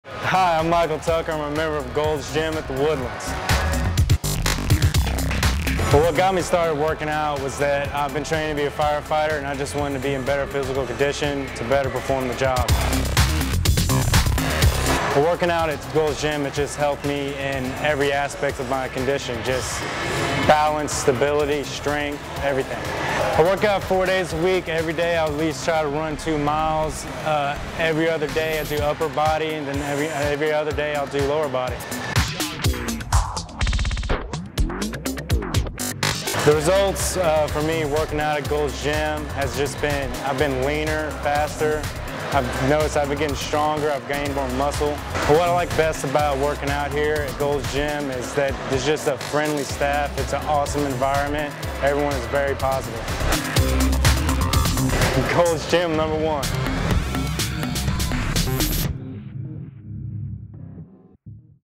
Success Story - Michael Tucker
Michael comes to Golds Gym Houston to prepare himself for becoming a fire fighter. Since he's started working out with a personal trainer at Gold's he's seen several improvements beyond weight loss and muscle gain.